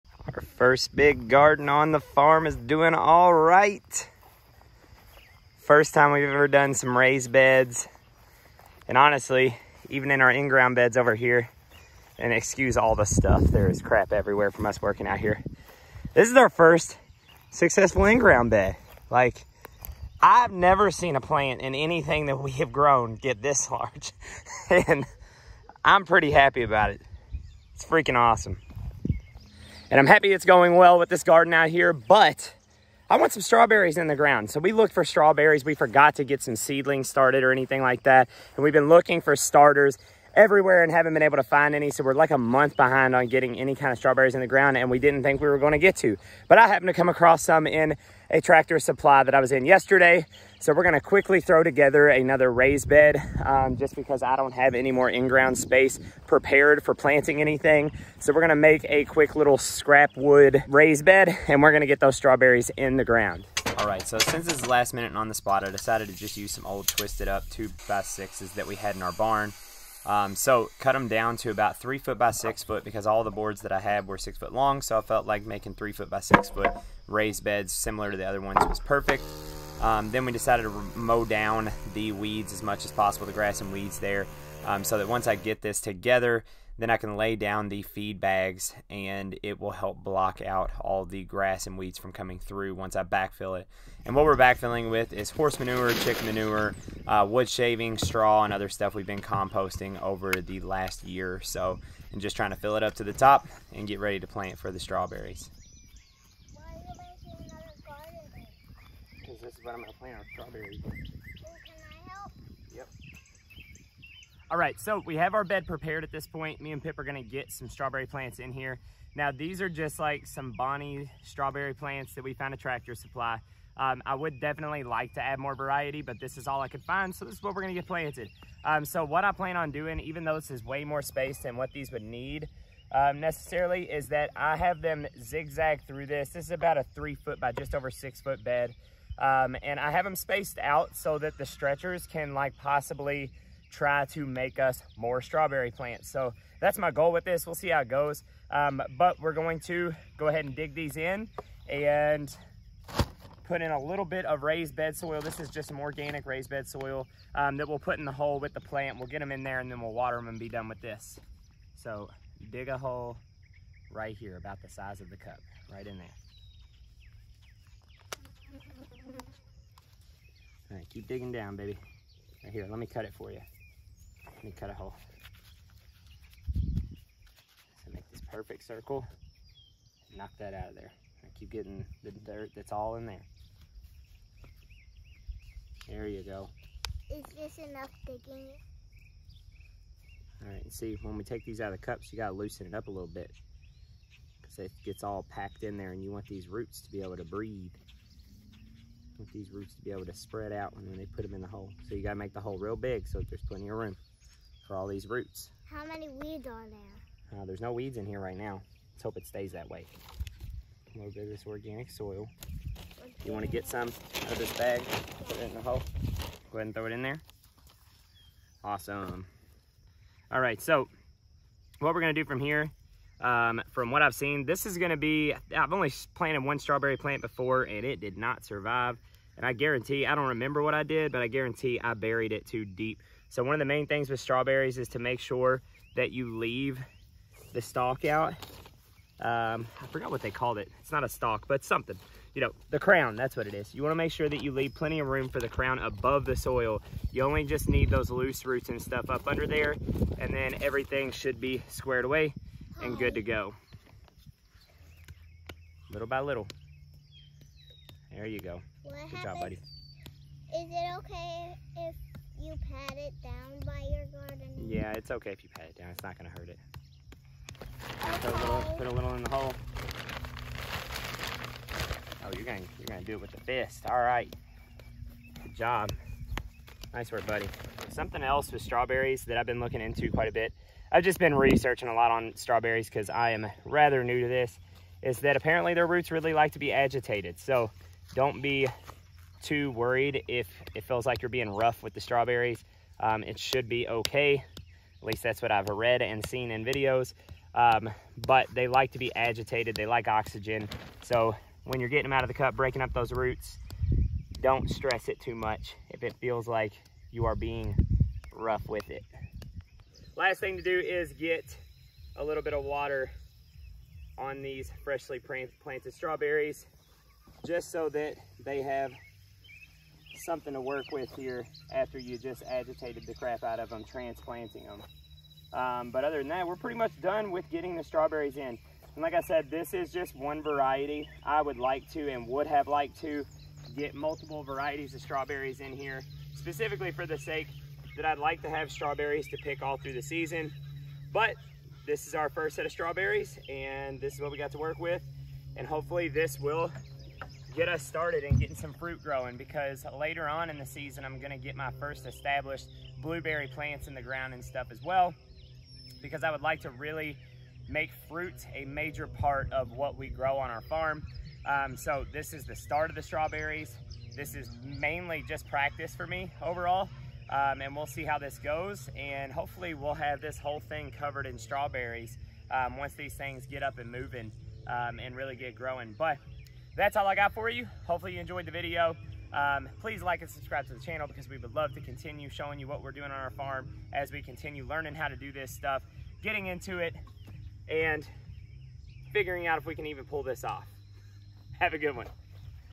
🍓Quick and Easy Raised Bed Prep For Our First Strawberries🍓
We are excited to share with you our very first strawberry garden planting! We hope to inspire you to take on your own garden adventure and reap the delicious rewards with your family and friends even if you get a bit of a late start like us.

Materials we use all in one place on our Amazon Storefront